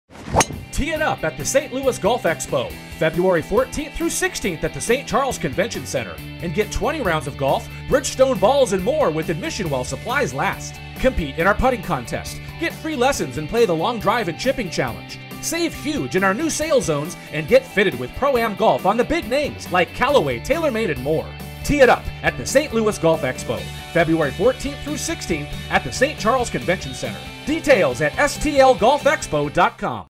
St. Louis Golf Expo 2014
30 second tv spot for the 2014 St. Louis Golf Expo